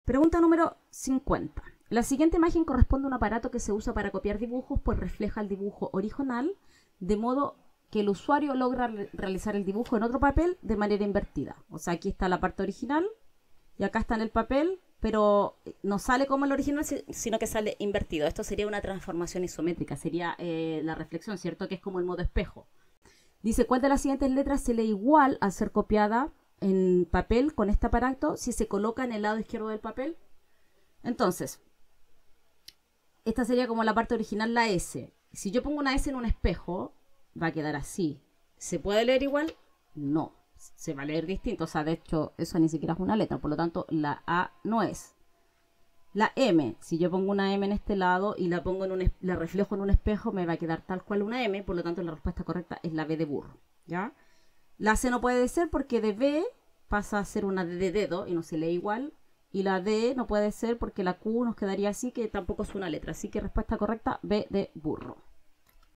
"RESOLUCIÓN PAES M1" Invierno Oficial 2024, pregunta N°50
PAES M1 Invierno Oficial 2024, pregunta N°50  PAES Matemáticas 1 de Invierno Oficial Admisión 2024, proceso 2023, para que puedas prepararte de mejor forma para la PAES.